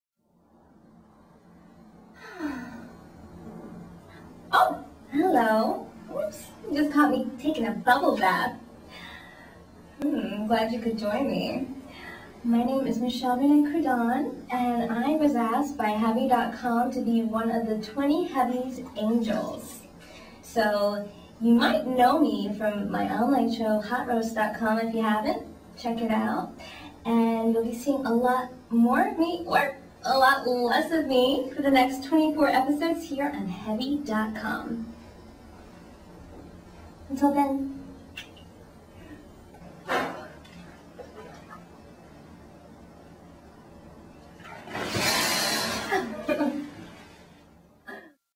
SEXY BUBBLE BATH FUN
Who doesn't love a bubble bath?? This is a promo spot I did for Heavy.com and has since inspired several parodies. Enjoy!